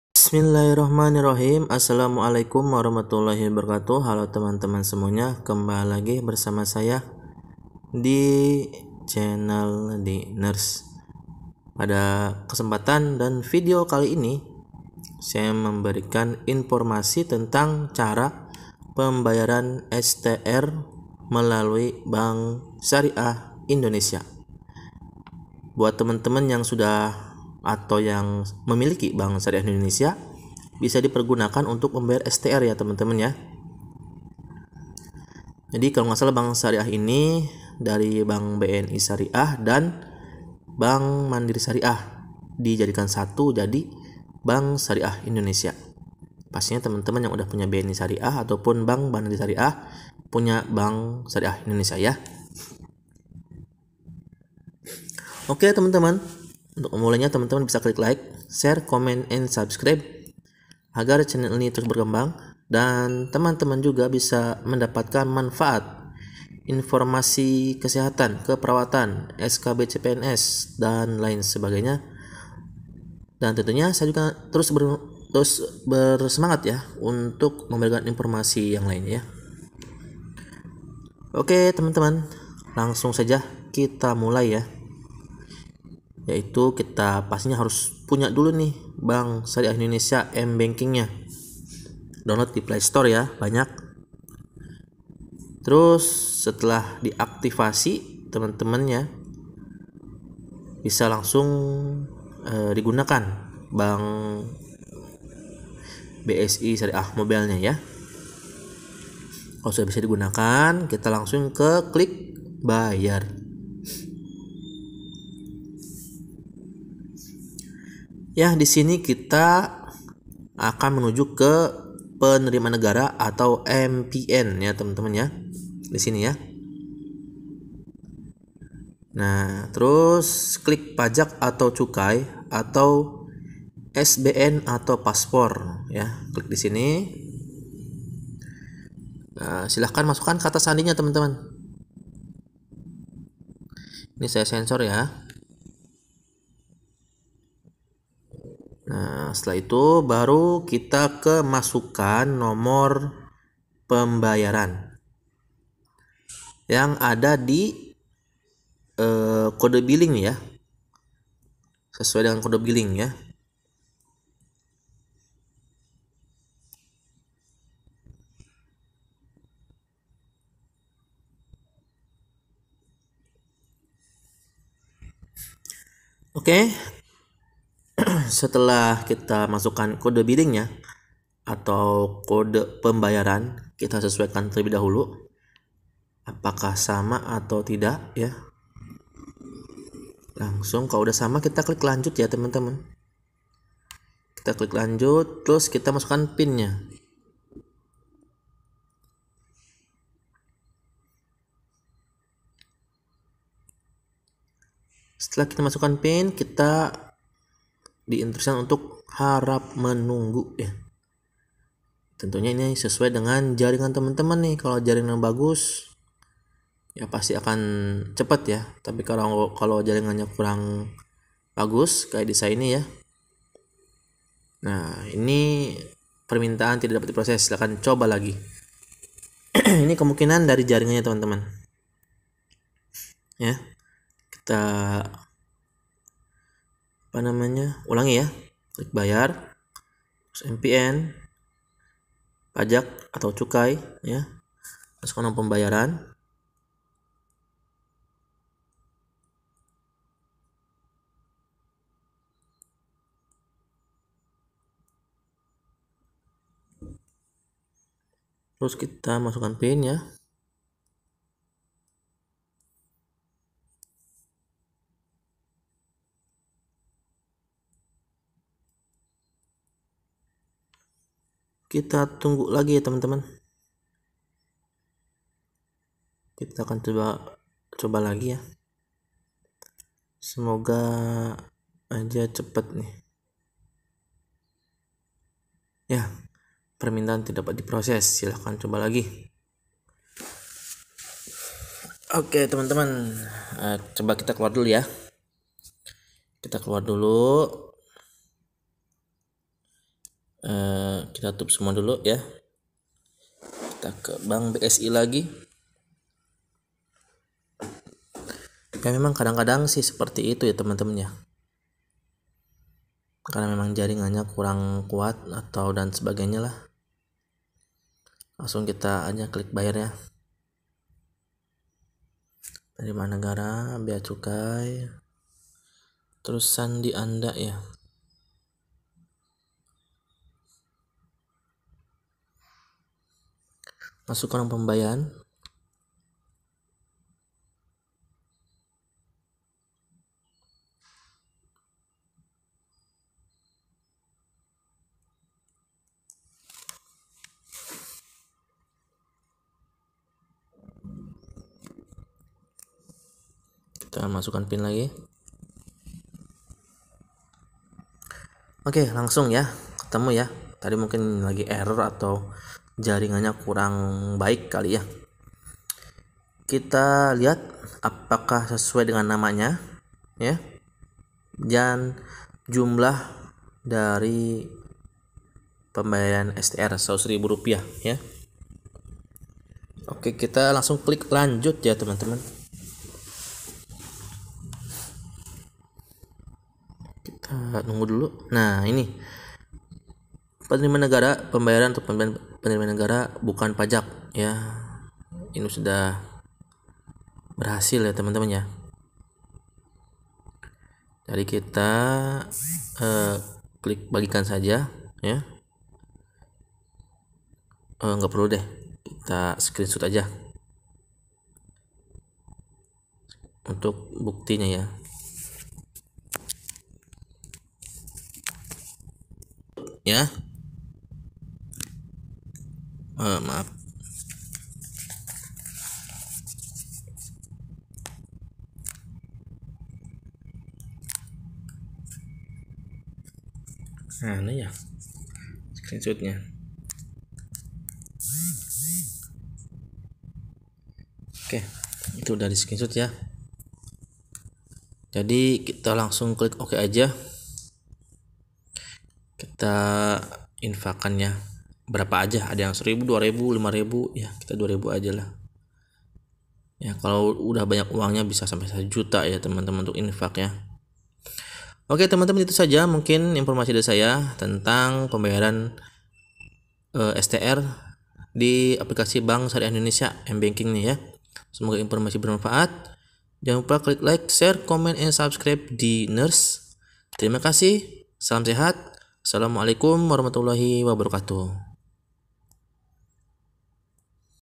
Cara Pembayaran STR Terbaru
pada video kali ini saya memberikan tutorial tentang cara pembayaran STR Melalui M-banking BSI (Bank Syariah Indonesia)